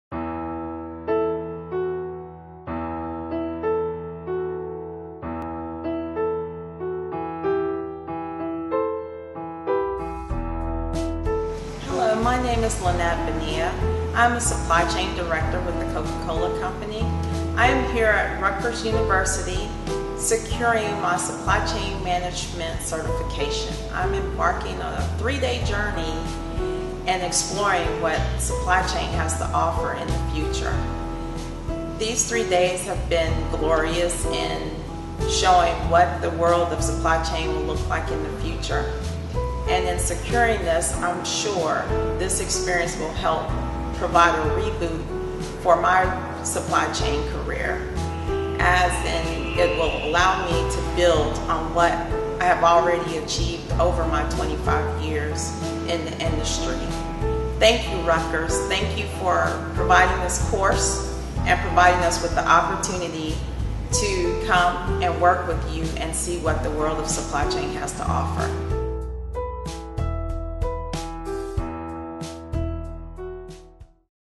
Rutgers 2018 Supply Chain Management Certification Program
Our exciting SCM Certification program covers the major aspects of supply chain management, including strategy, sourcing and procurement, logistics, operations and organizational alignment. Learn how to execute a strategic supply chain management plan that is grounded in today’s business needs for competitive effectiveness and operations efficiency.